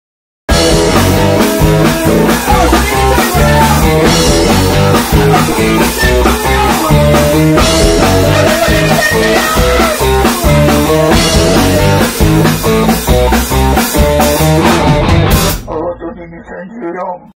アバソニ2014
2014年9月6日
イベントタイトル「アバソニ！2014」
本町Mother Popcorn

出演
1. SOUND☆MAN（17:30～18:15）
2. GOLD DATES（18:25～19:10）
3. ICE FIRE（19:20～20:05）
4. ABBα（20:15～21:00）

料金￥1,000（入場時別途ドリンク代￥500要）